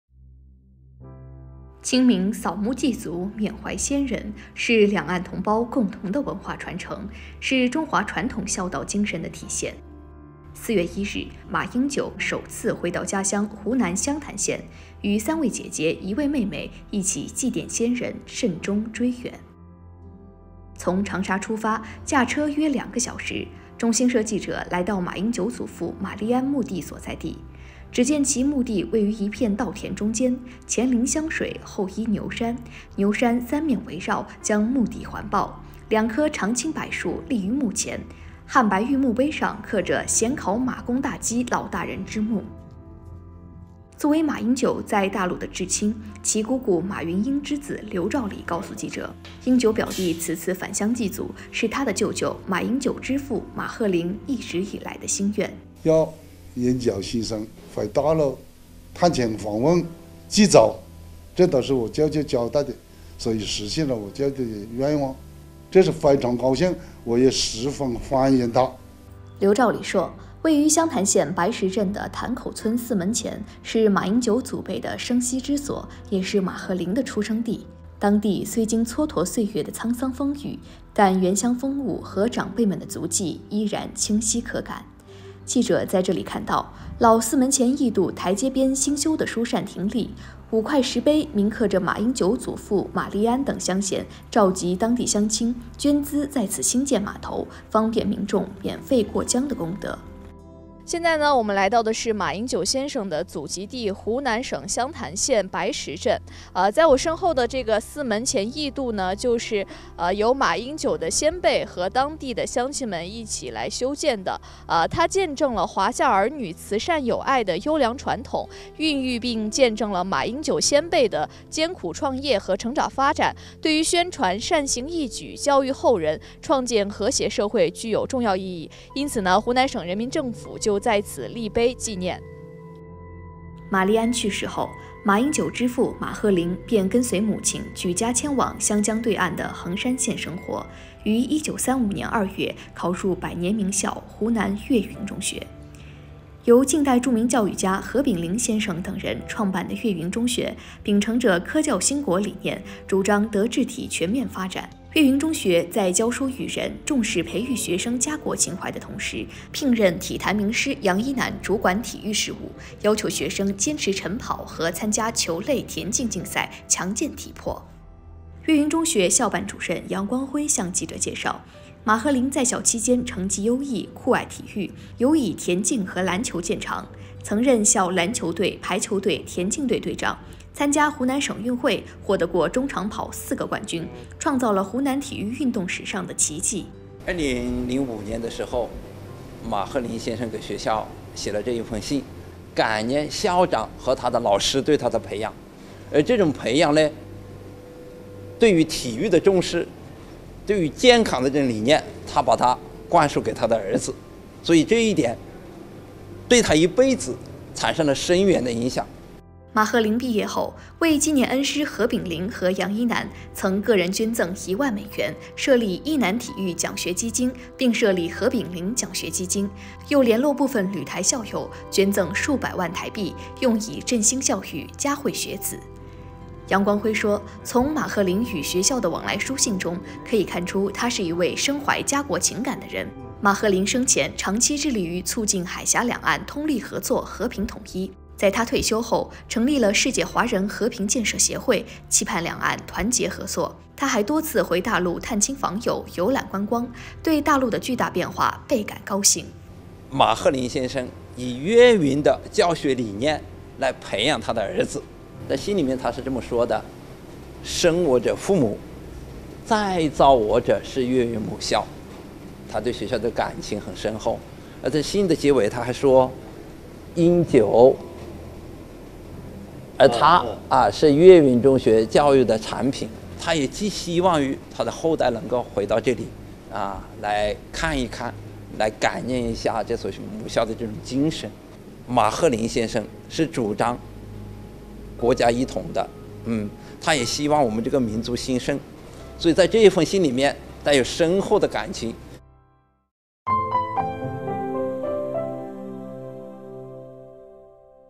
寻访马英九祖籍地：深厚文化底蕴培育浓烈家国情怀
清明扫墓祭祖缅怀先人，是两岸同胞共同的文化传承，是中华传统孝道精神的体现。4月1日，马英九首次回到家乡湖南湘潭县，与3位姐姐、1位妹妹一起祭奠先人、慎终追远。
从长沙出发驾车约两个小时，中新社记者来到马英九祖父马立安墓地所在地。只见其墓地位于一片稻田中间，前临湘水后依牛山，牛山三面围绕，将墓地环抱。两颗常青柏树立于墓前，汉白玉墓碑上刻着“显考马公大基老大人之墓”。
作为马英九在大陆的至亲，其姑姑马云英之子刘肇礼告诉记者，英九表弟此次返乡祭祖，是他的舅舅——马英九之父马鹤凌一直以来的心愿。